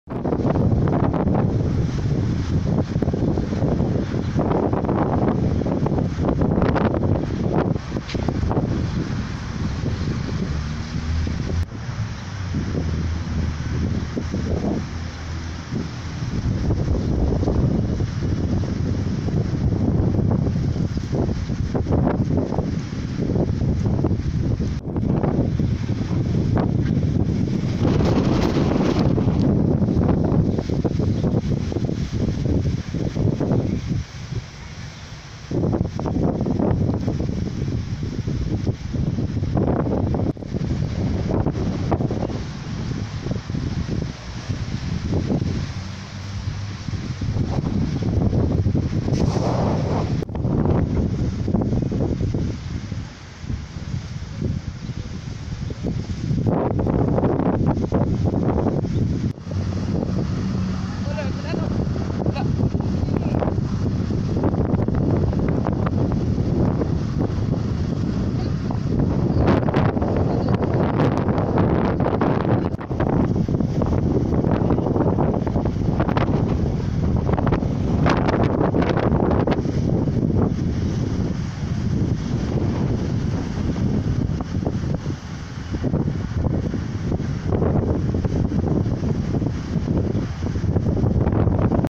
Así llegaba este mediodía, el temporal a la ciudad de Concordia
El Servicio Meteorológico ratificó el alerta. No obstante, las condiciones empezaron a mejorar en el oeste de Entre Ríos, incluida la capital provincial. El este de la provincia y Uruguay, eran este mediodía las zonas más afectadas por las la tormenta.